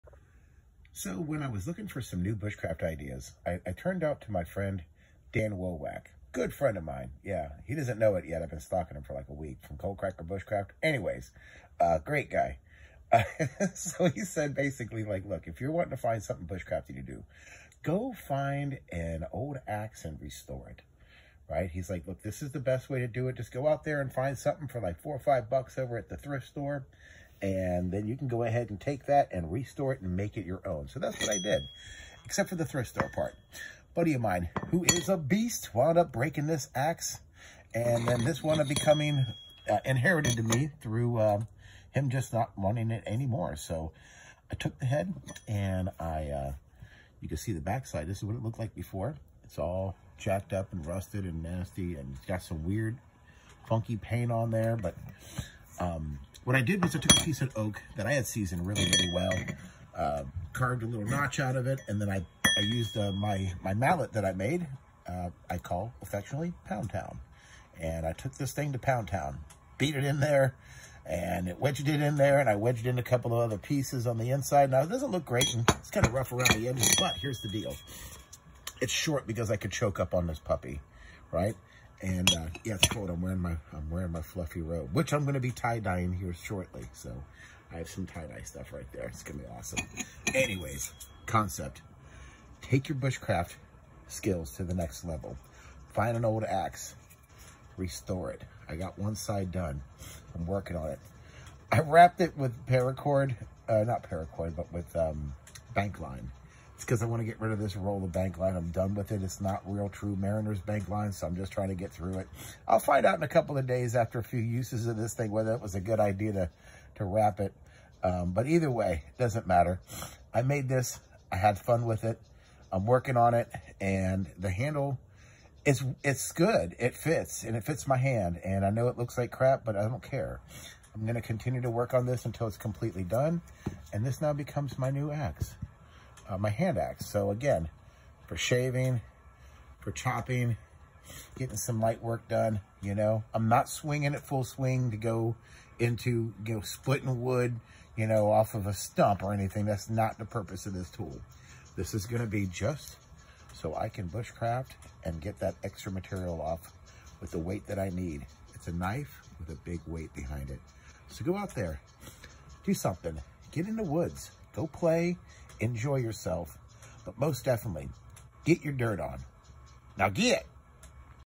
Axe head restored into a new tool
Buddy of mine left this axe head after he broke the handle. It’s been sitting around for a while now and I figured good steel shouldn’t go to waste. This was my version of restoring this busted axe.